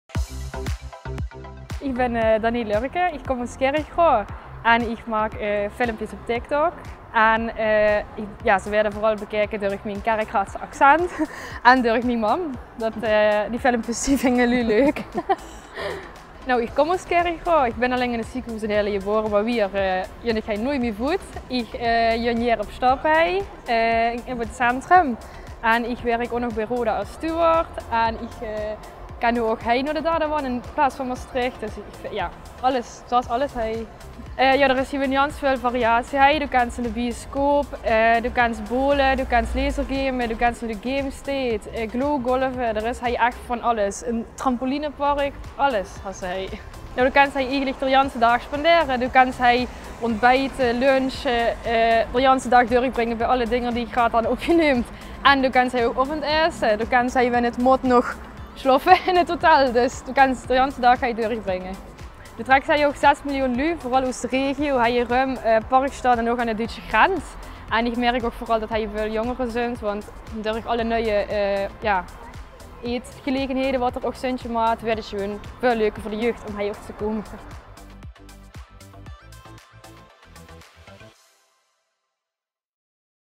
Het Kerkrade van Danee Lurken
Danee is een echte Kerkraadse en maakt in haar vrije tijd TikTok filmpjes die veel bekeken worden door haar Kerkraadse accent én haar moeder. In deze Story vertelt ze wat er allemaal te beleven is in Kerkrade. Kijk mee!